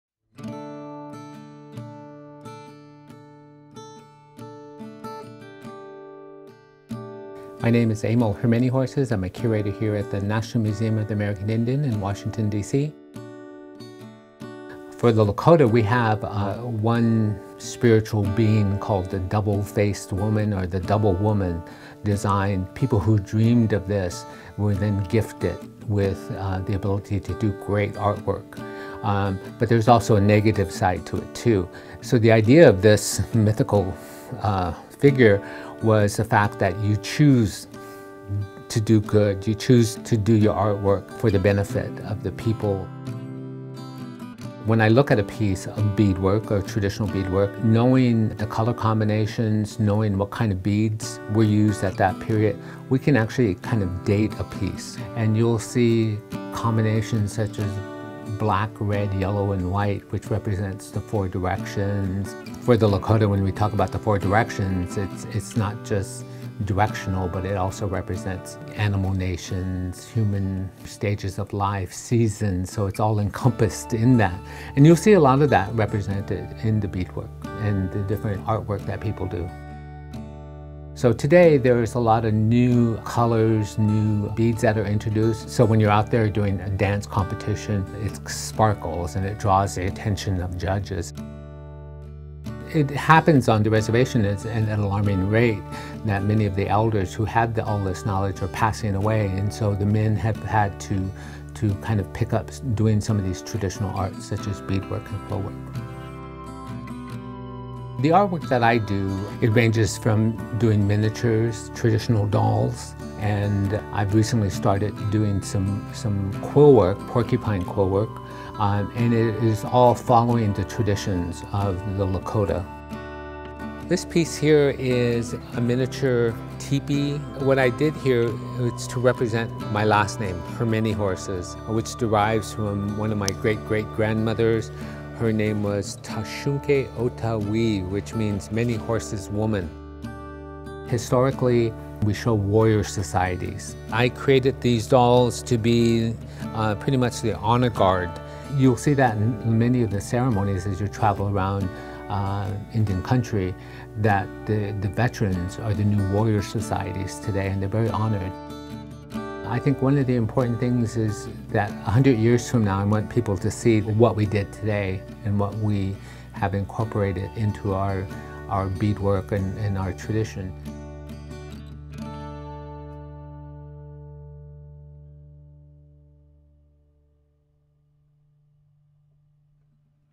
Emil Her Many Horses
Emil Her Many Horses is recognized as an accomplished artist in contemporary beadwork and contemporary dolls who specializes in Northern and Southern Plains culture. Emil was born and raised on the Rosebud Sioux Indian Reservation in South Dakota. He is an Associate Curator at the Smithsonian’s National Museum of the American Indian (NMAI) in Washington, DC.